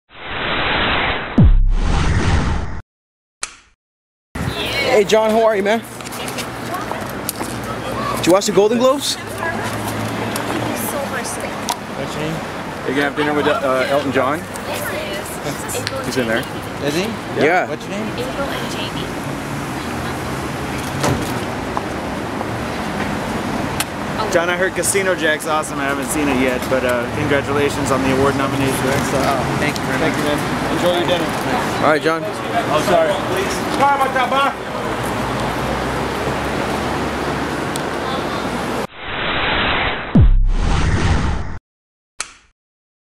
Jon Lovitz Signs Autograph at Boa
Arriving at BOA dressed casually and apparently in an introspective mood,  Casino Jack actor Jon Lovitz signs an autograph before cruising into the restaurant.